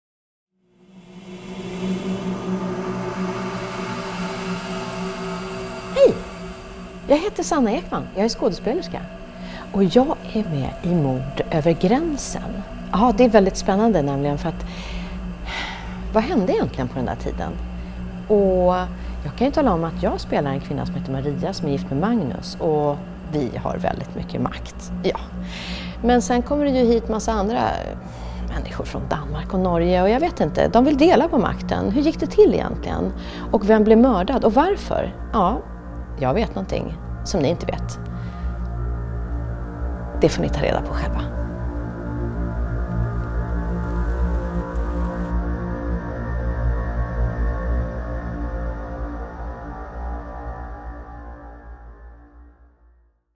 Intervju med skådespelerskan Sanna Ekman inför Mord över gränsen
Presentation av skådespelare till Mord över gränsen 1397.
(26 mars Livestream)